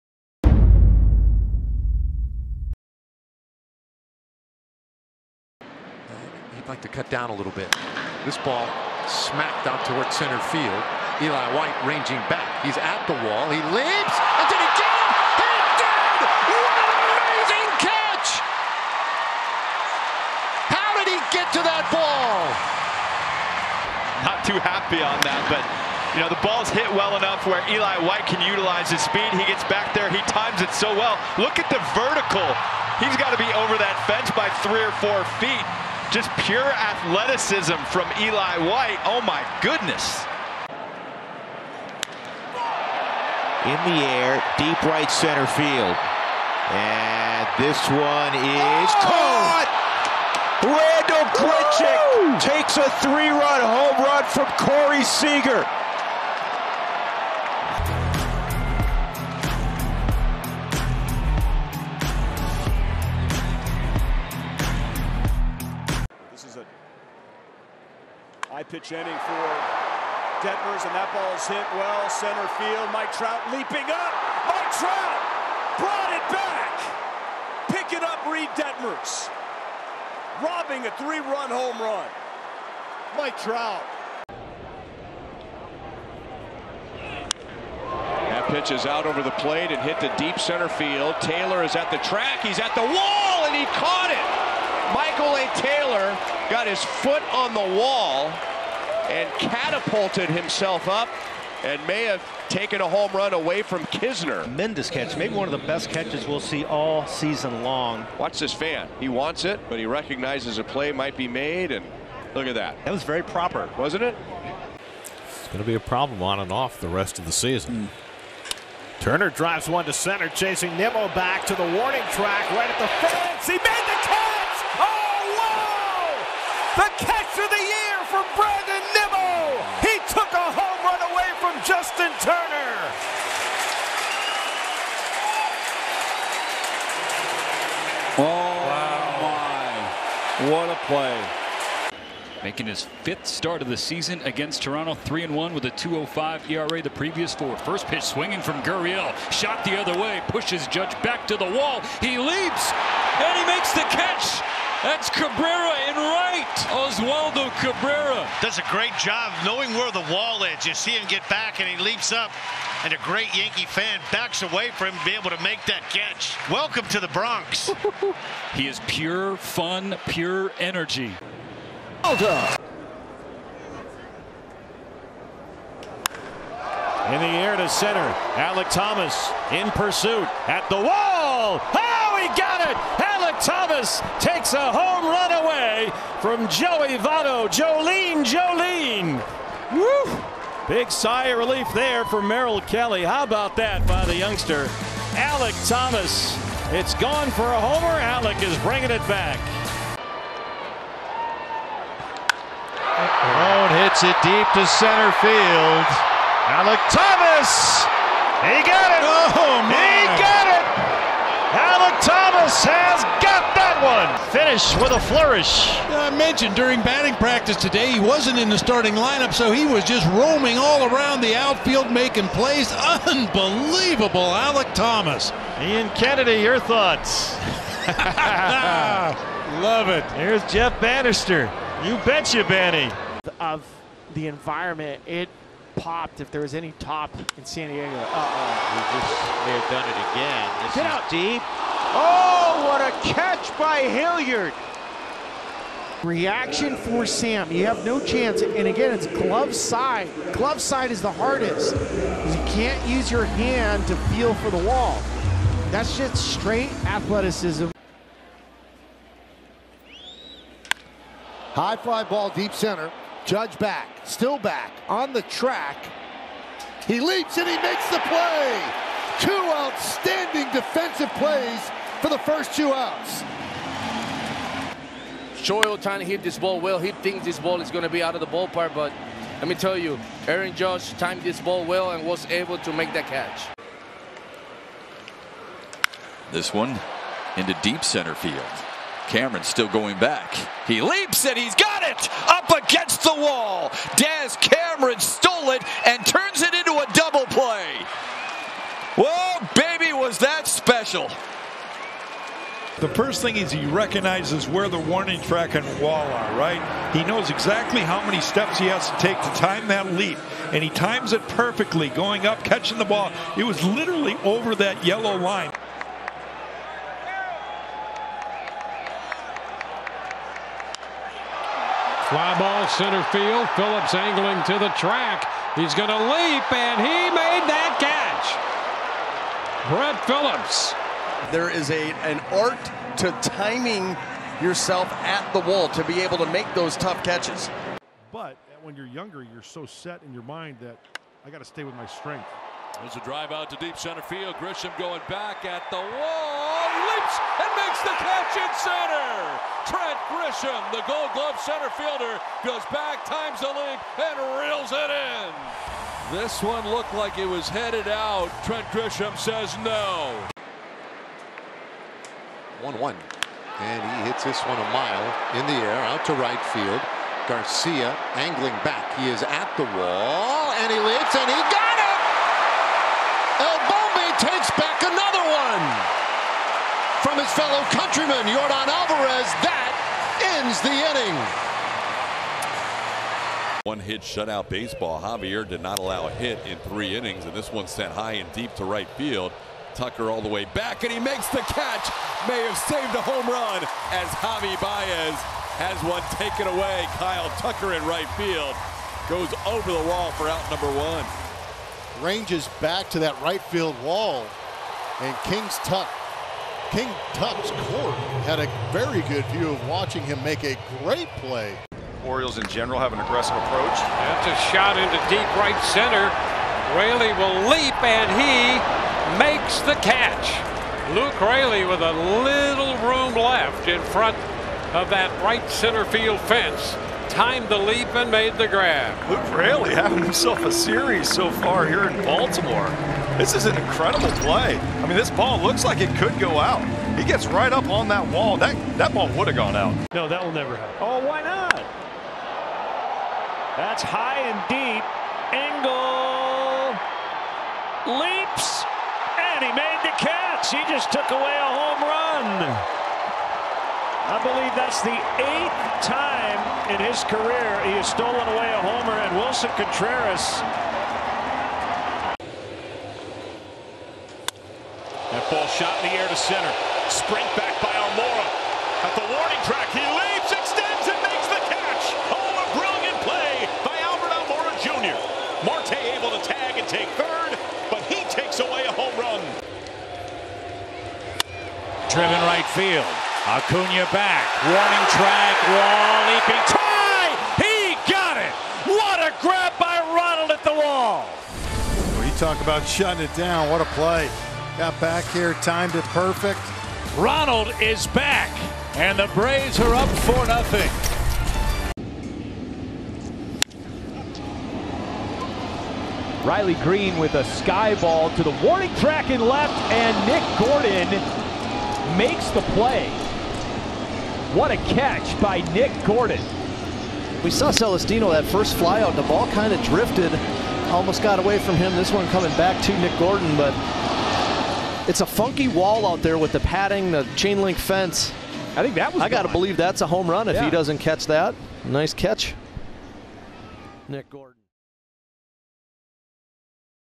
MLB Robbing Home Runs 2022
The best home runs robs of the 2022 MLB season.